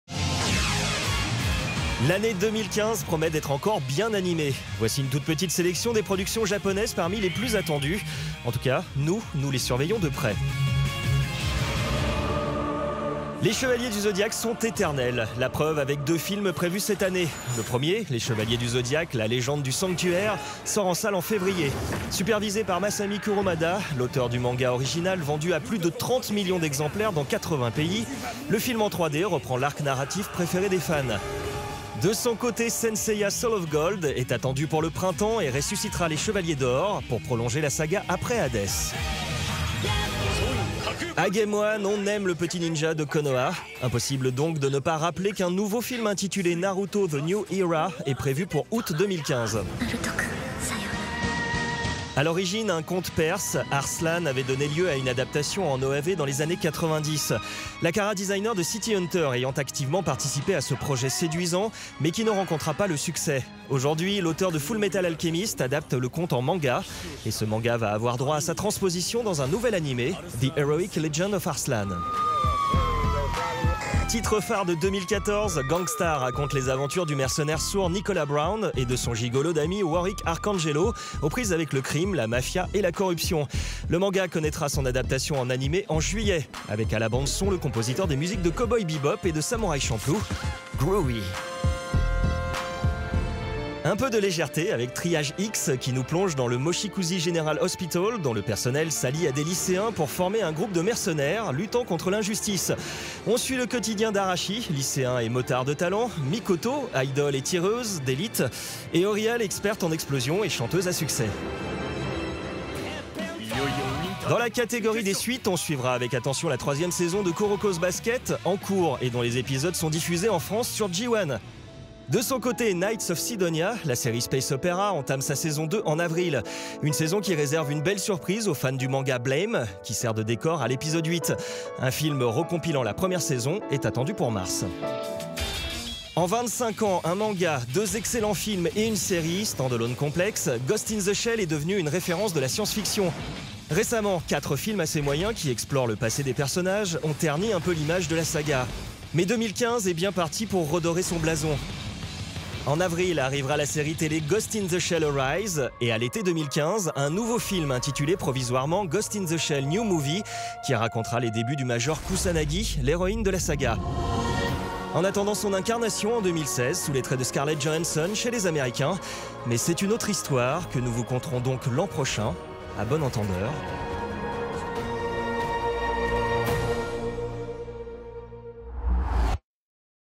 Les animes à suivre en 2015 !
Regardez notre sélection des animés Japonais les plus attendus en 2015 !